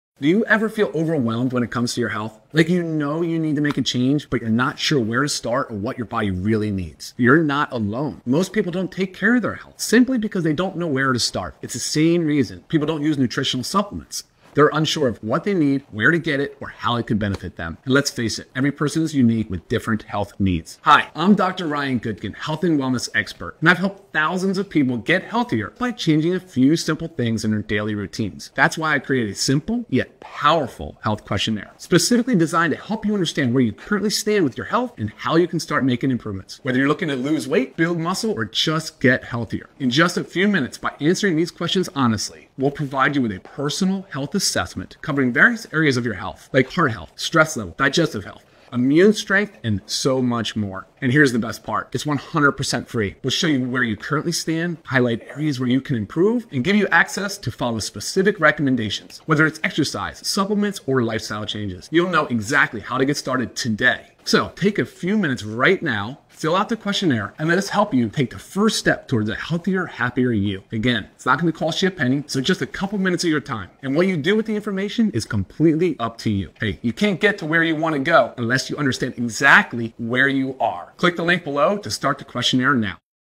Health and wellness questionnaire, FREE!!
Watch for details of health and wellness questionnaire, it's FREE and the Doctor will send you an honest appraisal and recommendations